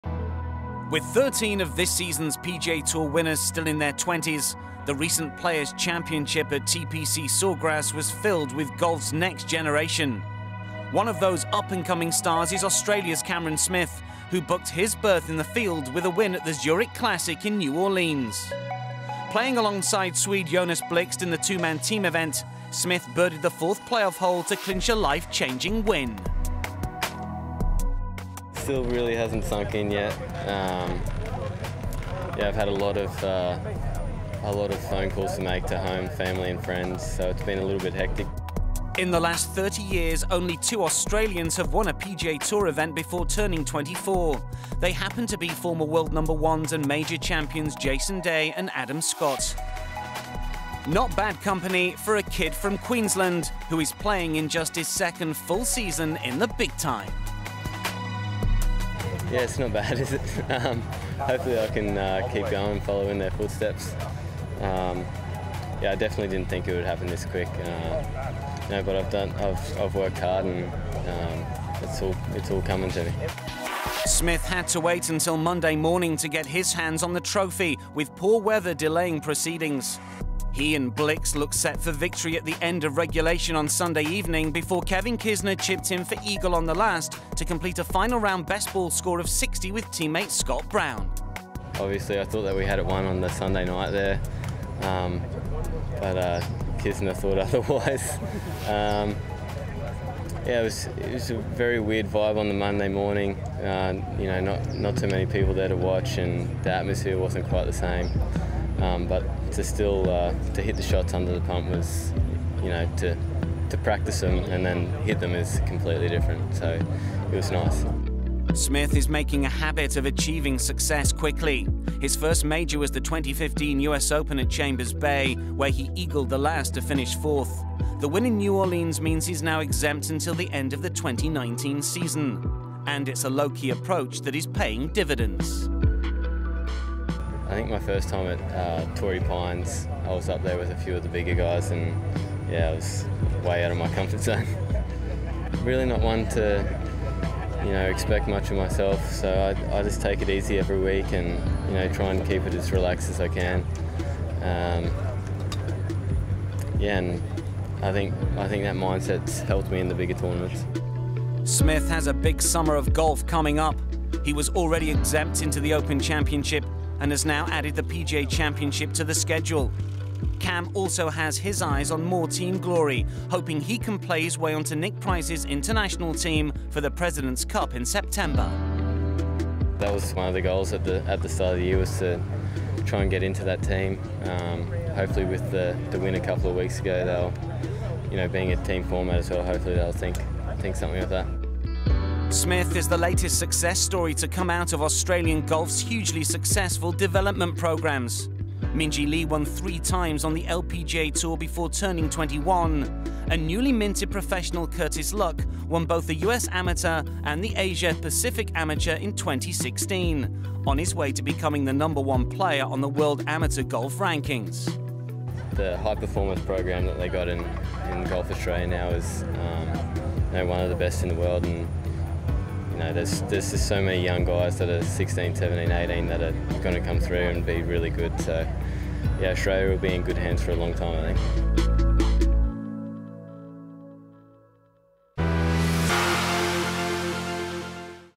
Player Profile: Cameron Smith
Rising Australian star Cameron Smith chatted to us recently at THE PLAYERS Championship about his and Jonas Blixt’s dramatic triumph at the revamped Zurich Classic of New Orleans and the raft of golfing talent coming out of Australia at the moment